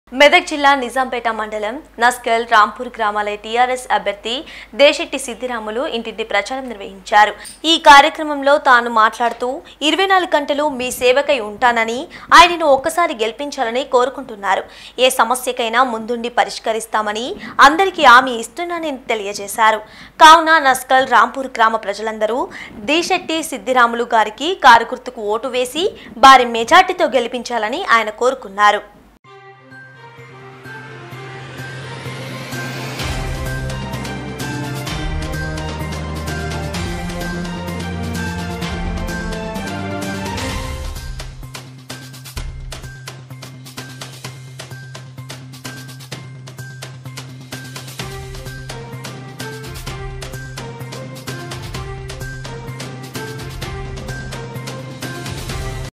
TS24 News నస్కల్,రాంపూర్ గ్రామం ఎంపీటీసీ అభ్యర్థి దేశెట్టి సిద్దిరాములు ఇంటింటి ప్రచారం
మండలం నస్కల్,రాంపూర్ గ్రామం ఎంపీటీసీ అభ్యర్థి దేశెట్టి సిద్దిరాములు ఇంటింటి ప్రచారం